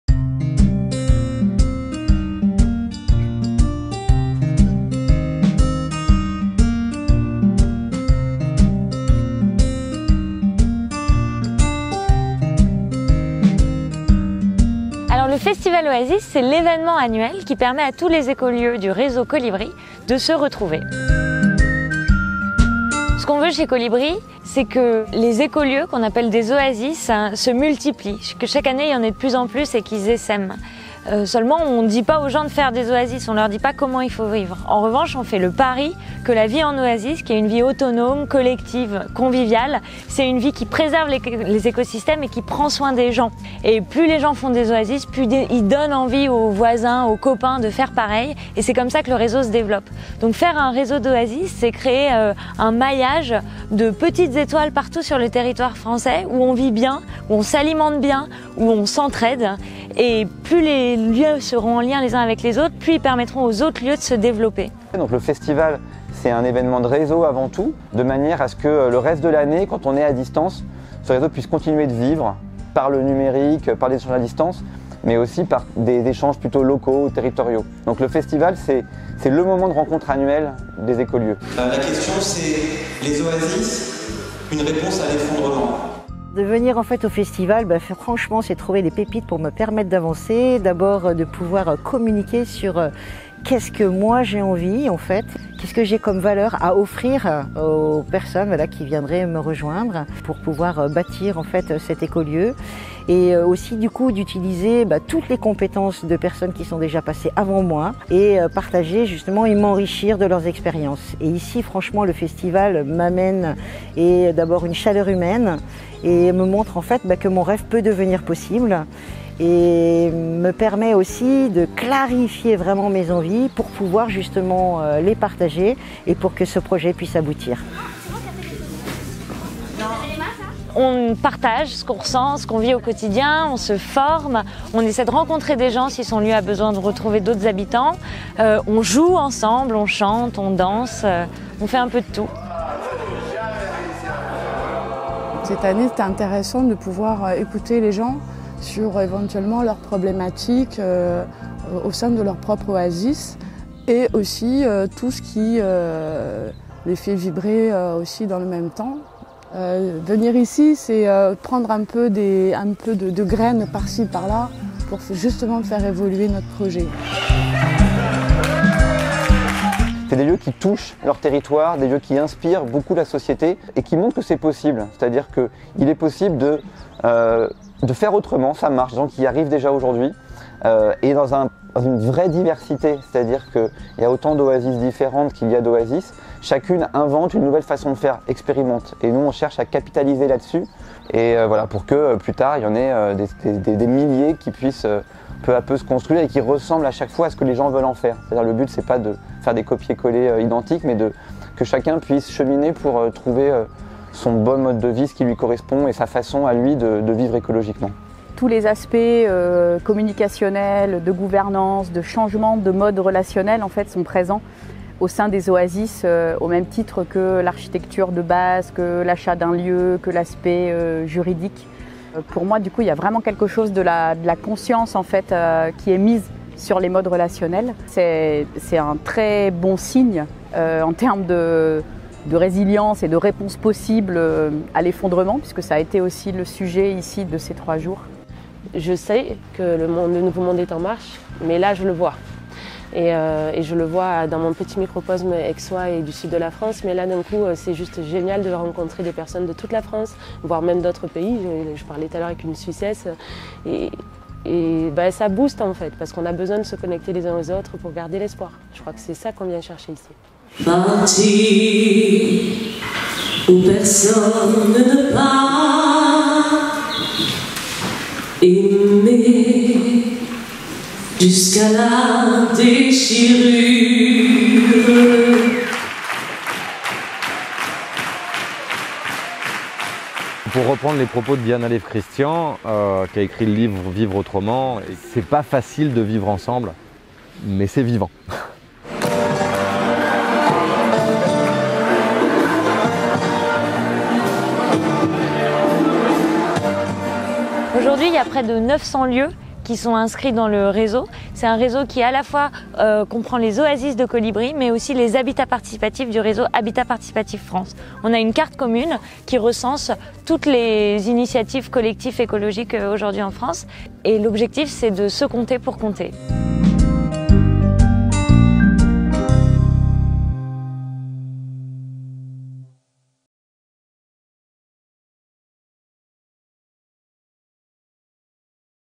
Le Festival Oasis 2019
A l’intérieur du parc du château de Jambville, se sont retrouvés en octobre 2019 une centaine d'habitants d'oasis, venus de la France entière. Reportage sur ce grand rassemblement des écolieux et des nouveaux modes de vie...

Vidéo montée par InProdTV - inprodtv.fr/